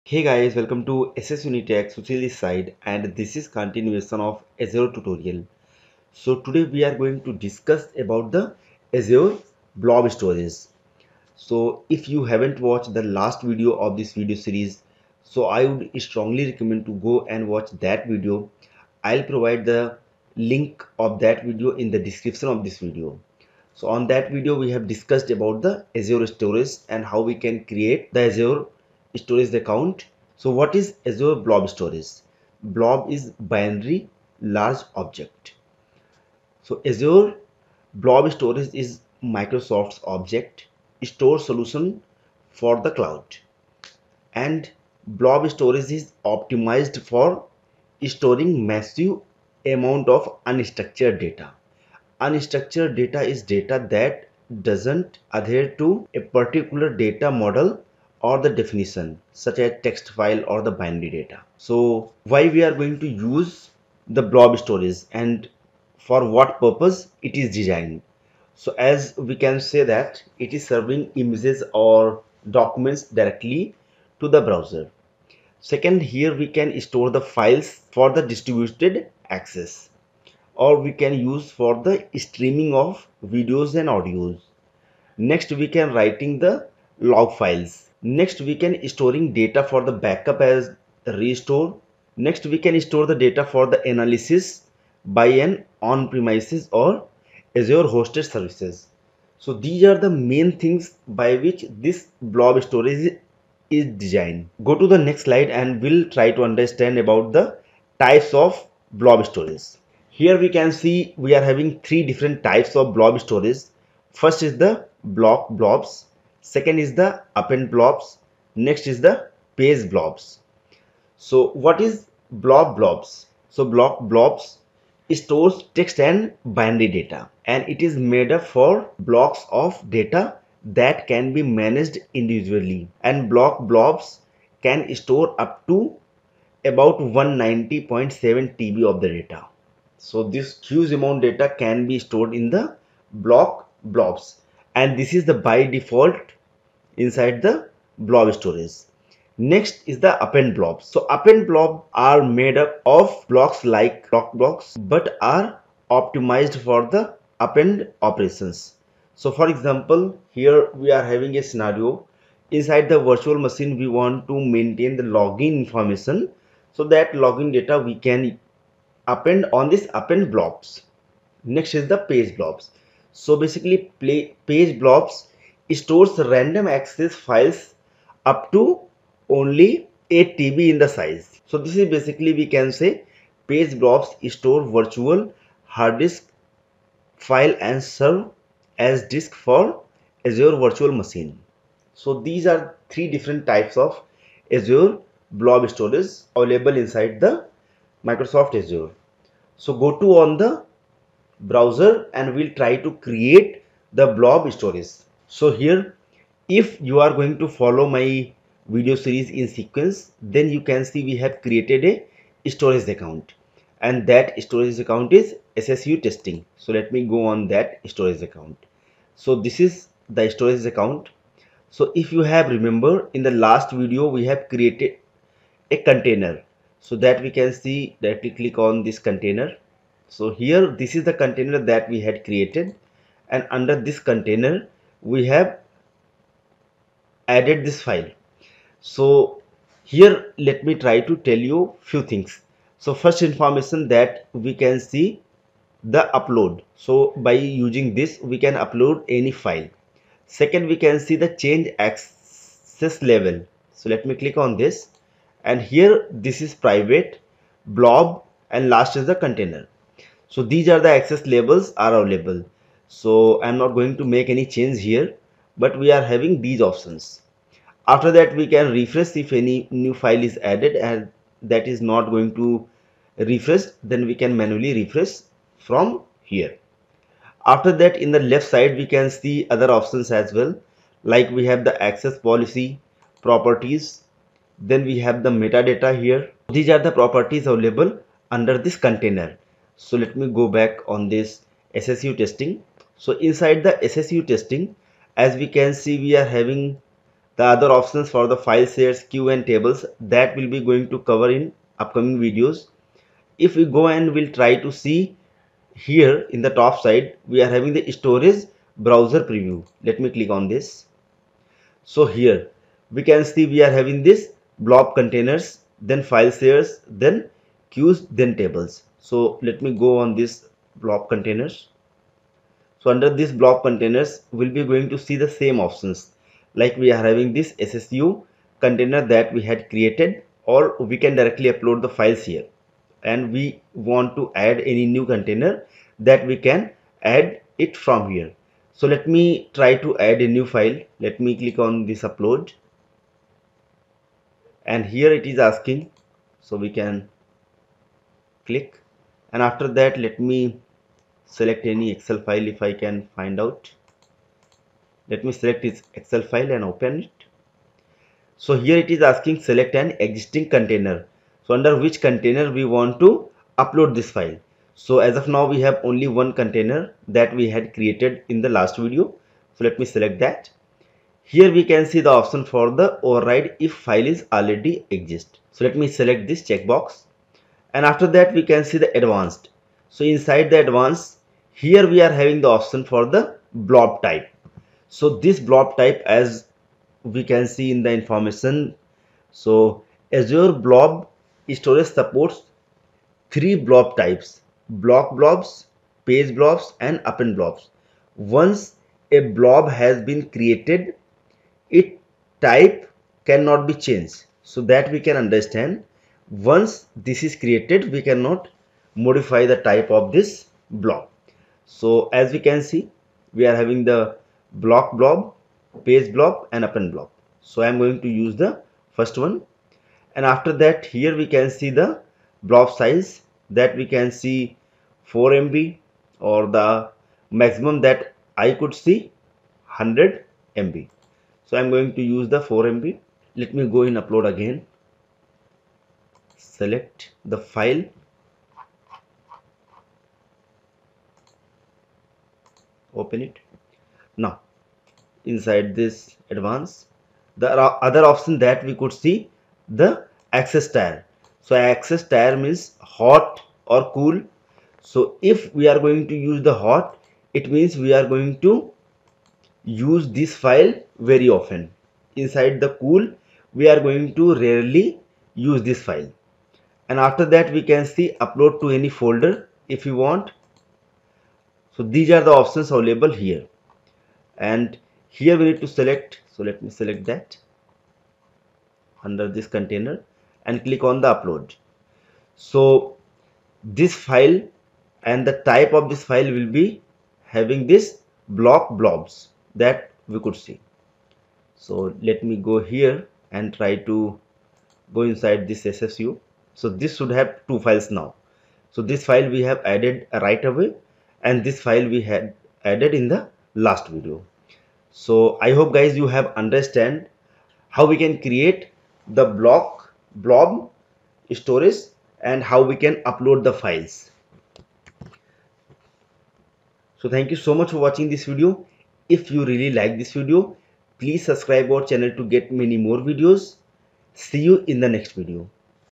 Azure blob storage tutorial | Block Blob, Append blob and Page Blob explained with demo
Here we have discussed about what is azure blob storage overview.